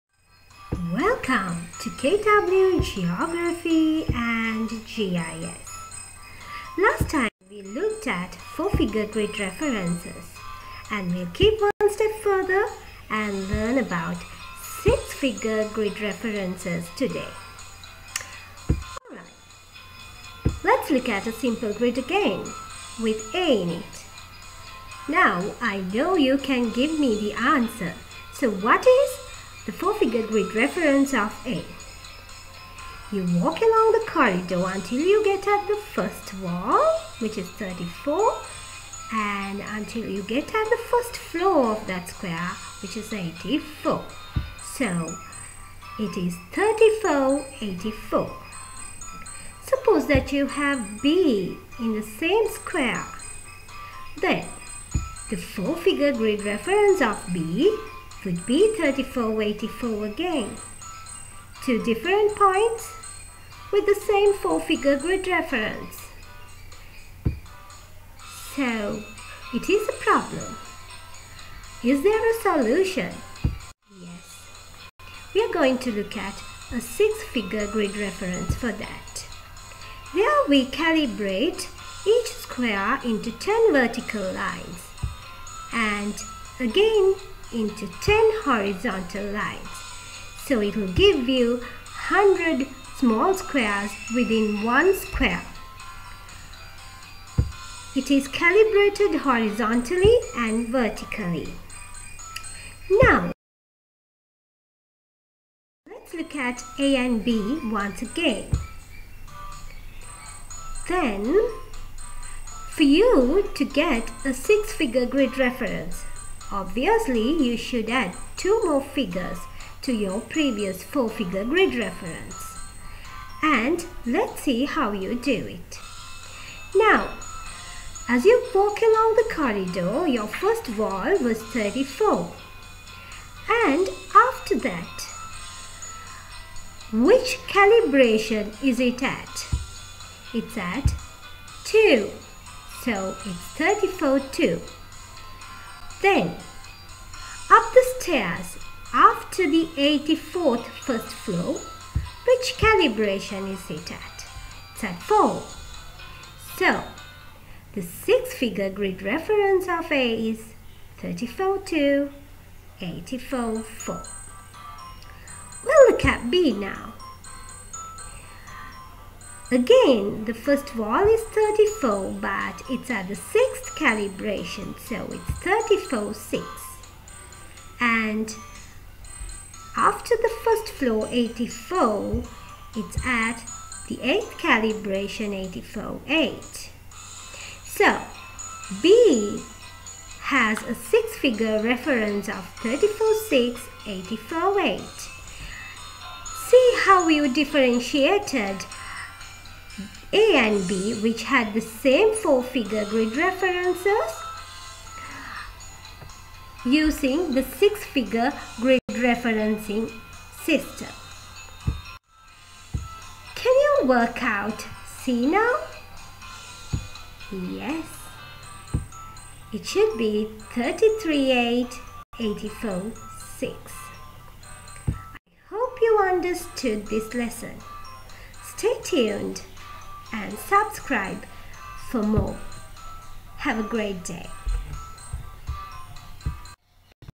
Six Figure Grid References
A short video designed with easy to learn and remember tip for students around the world, taking them one step further from the four figure grid references.
for the previous step.